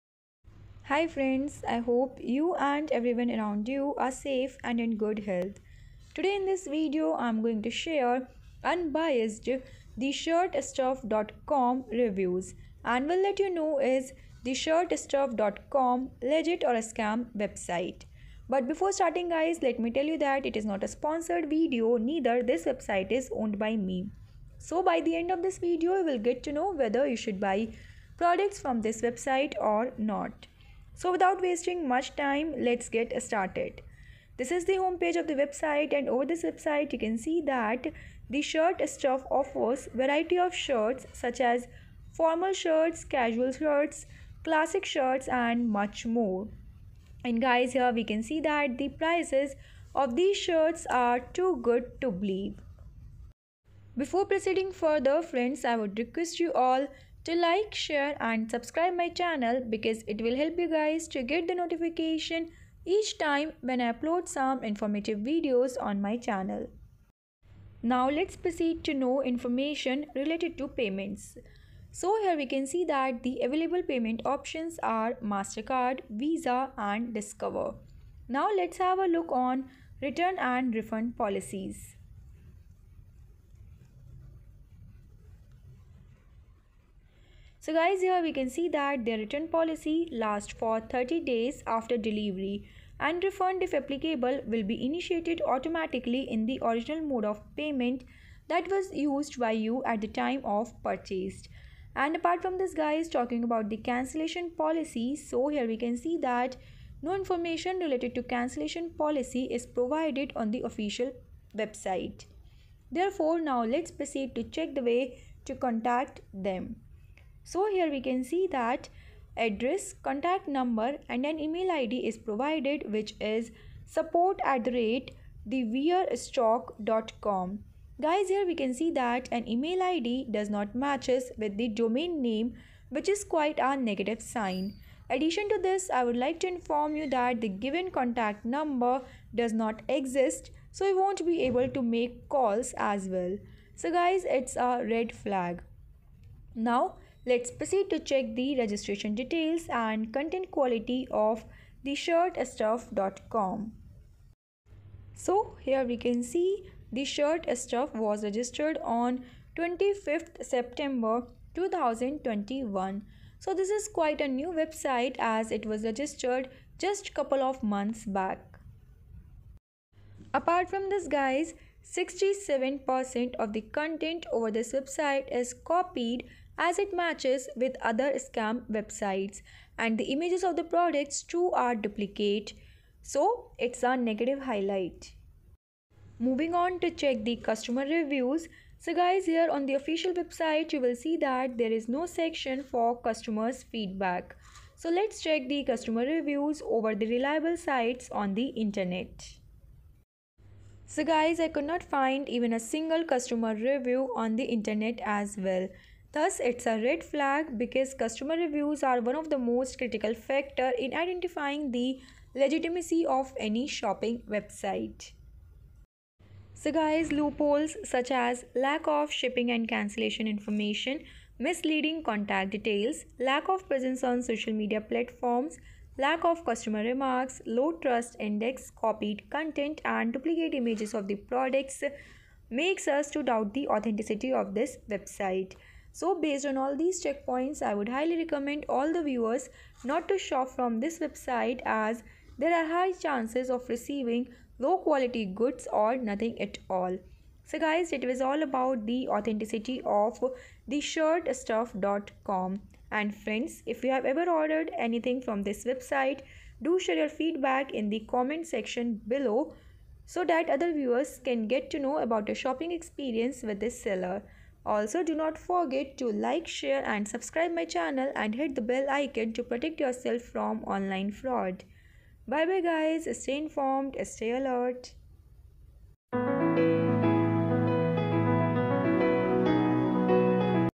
Is Theshirtstuff.com Genuine or Ploy | Watch Impartial Reviews on Theshirtstuff Shopping Website!
Hey Friends, today in this video I'm sharing Impartial Reviews on Theshirtstuff.com. Do Watch the video till end before you plan to buy products from this shopping website. I assure you that by the end of the video you will appreciate yourself for watching the video before reaching to final decision.

Office Address:- Near Market Sector 76 Surat, Opp.  Shoes Market, Surat - 395001

Scam Alert